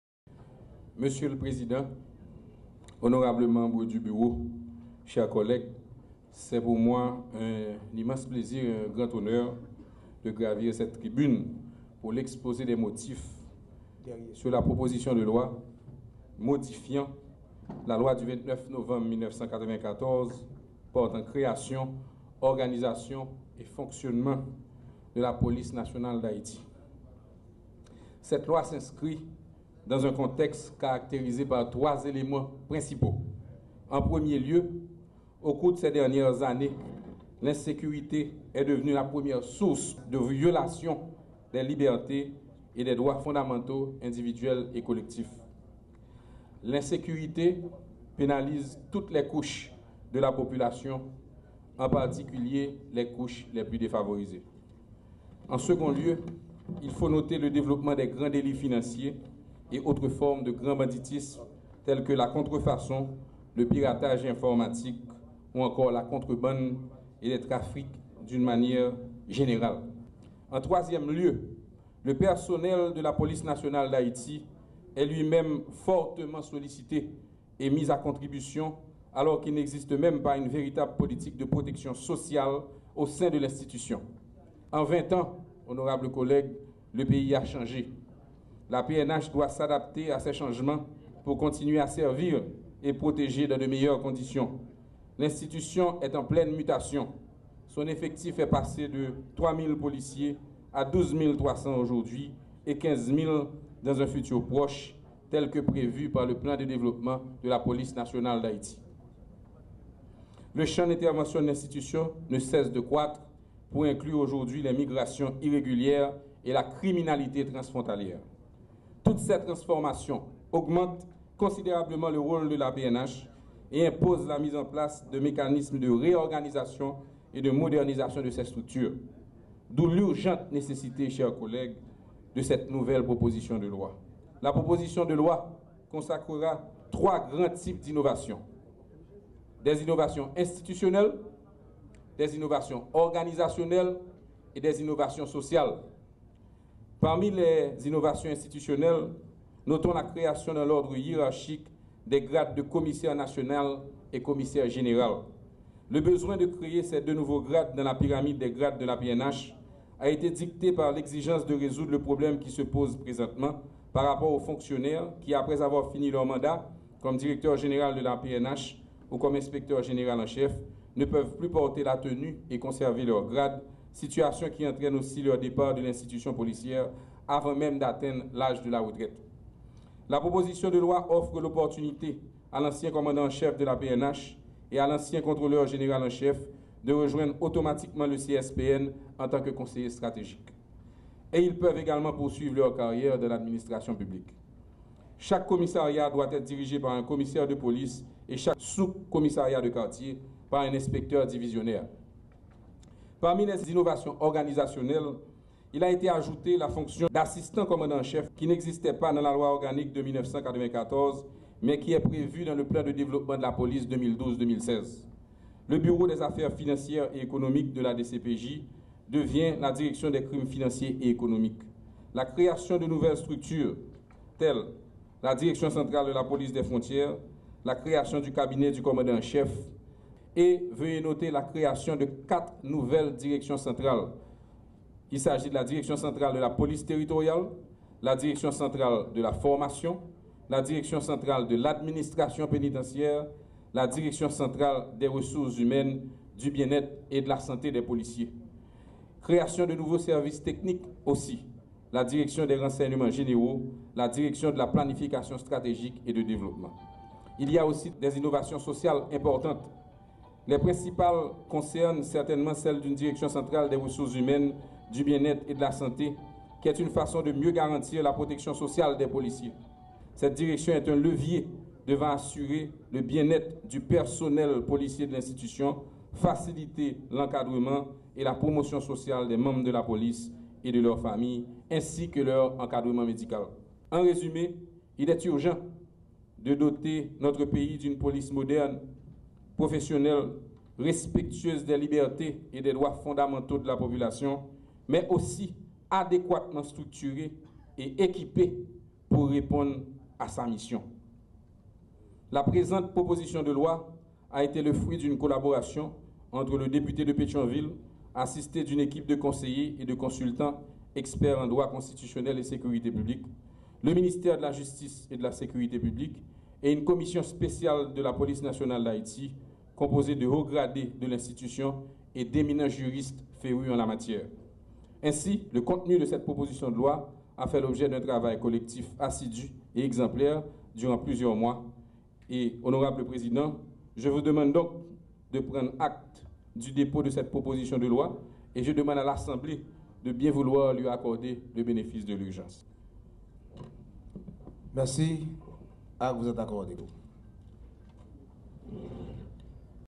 Jerry Tardieu dépose une nouvelle proposition de loi sur la Police Nationale d’Haïti (PNH)
En séance, à la chambre basse, ce 3 Mai 2016, Le Député Jerry Tardieu introduit une importante  proposition de lois sur la Police Nationale d’Haiti. Cette loi participe de l’urgente nécessité de doter le pays d’une Police moderne qui soit mise dans les meilleures conditions pour protéger et servir la population. La sécurité est, selon le député Tardieu, le pilier de toute société désireuse de préserver un climat propice à l’investissement, à la création d’emploi et à l’établissement d’un état de droit.